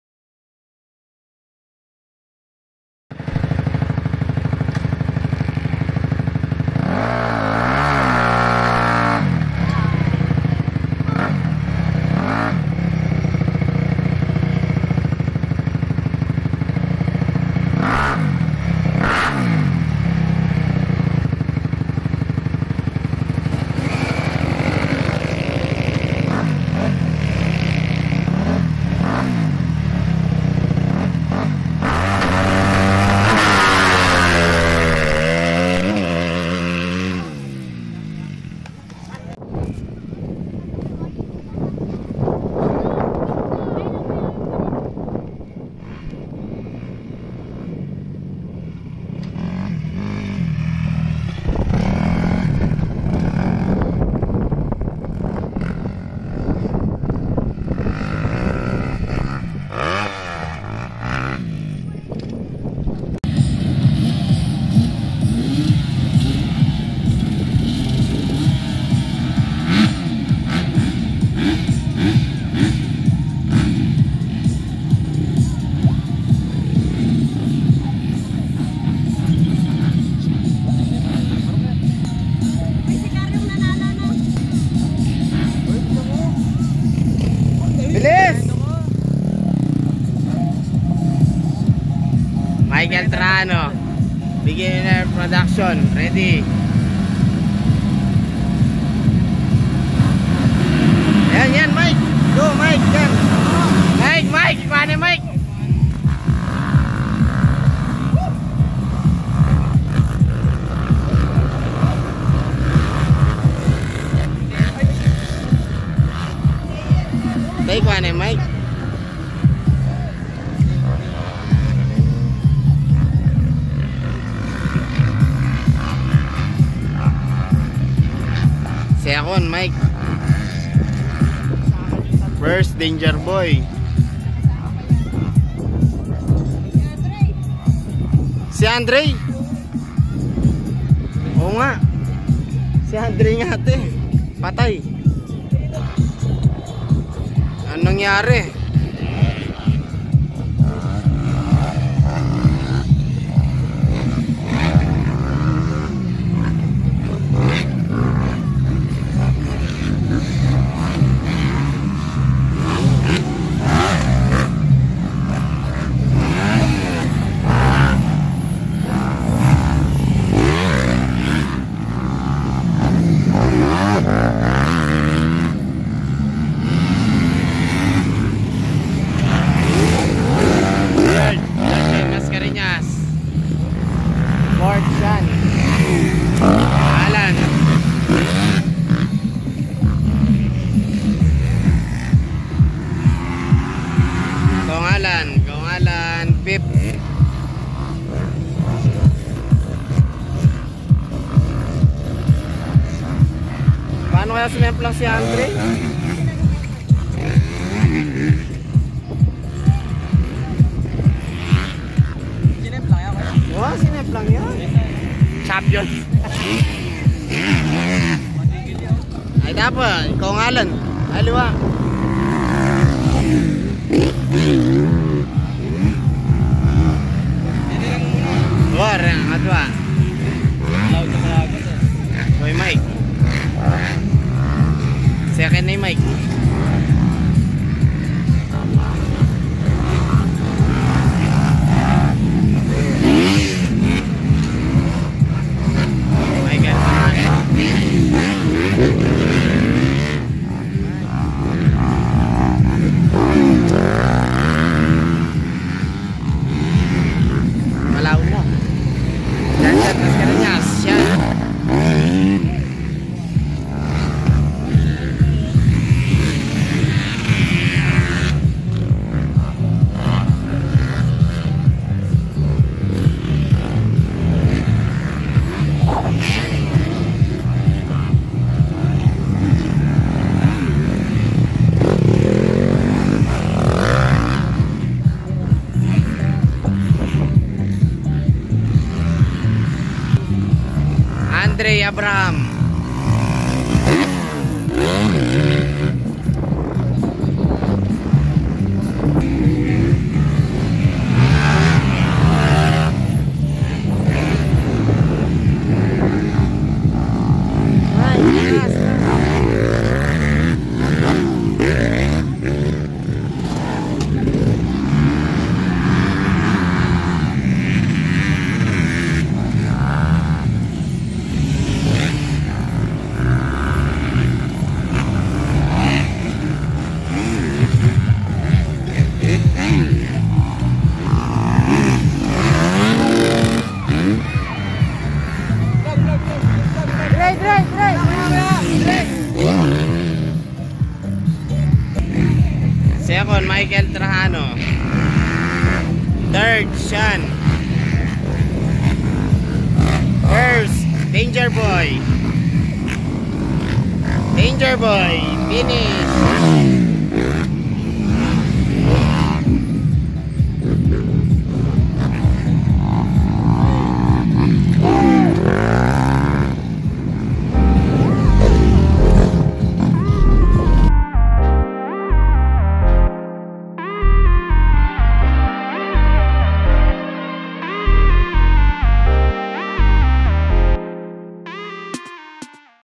laruk laruk festival motocross racing event by gov. ebdane of candelaria Zambales
8sevenmotorsports